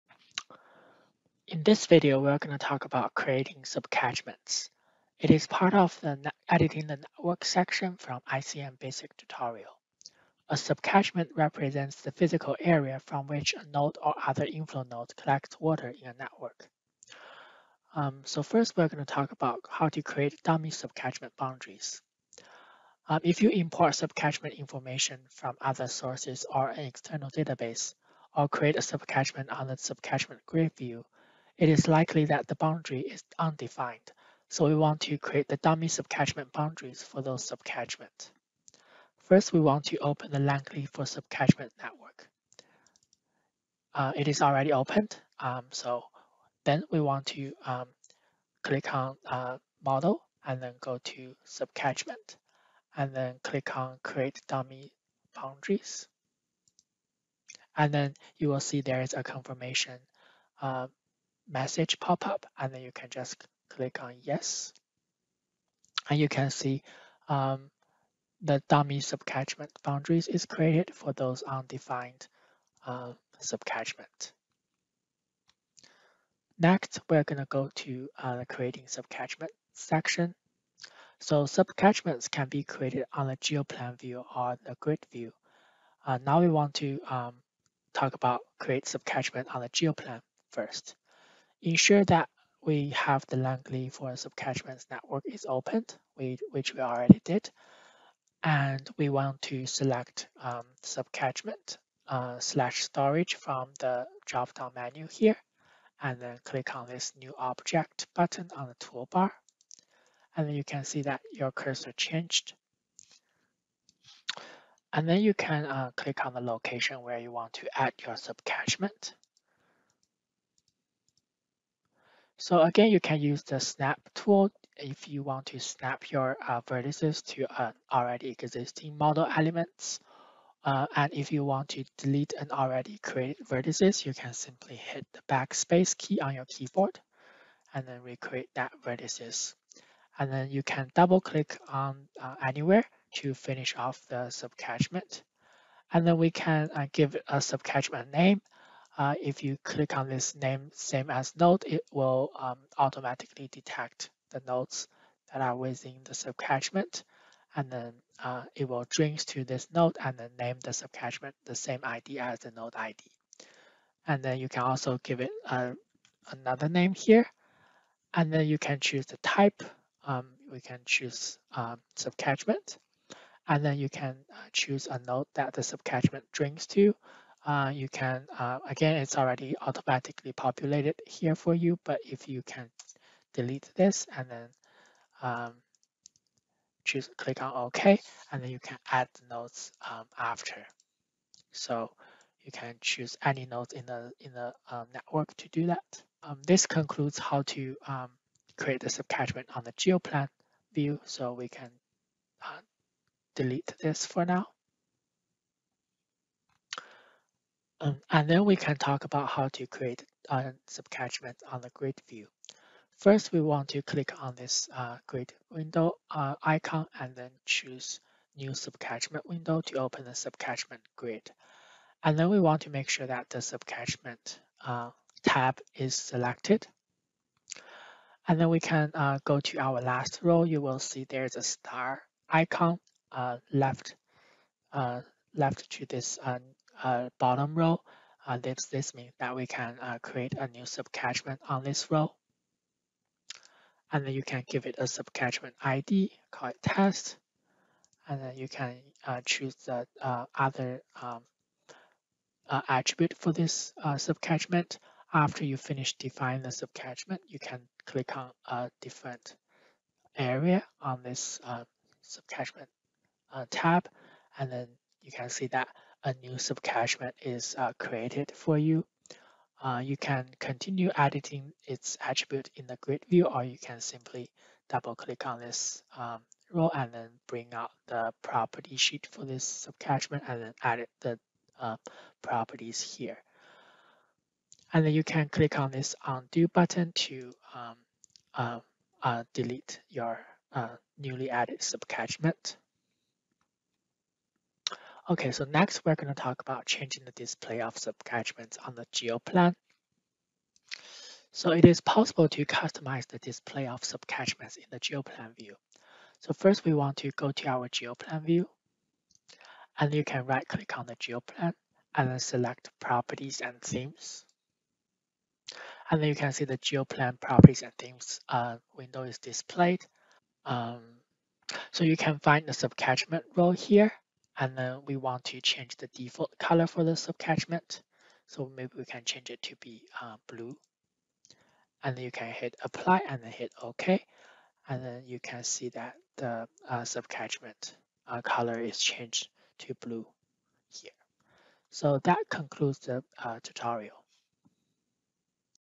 InfoWorks ICM Basics Tutorial Series | Video 13: Creating sub-catchments
This topic illustrates how to create dummy sub-catchment boundaries and how to create sub-catchments. A sub-catchment represents the physical area from which a manhole or other inflow node collects water in a network.

This entire tutorial series is also available on the One Water blog

Love to talk about what’s going on in the world of water?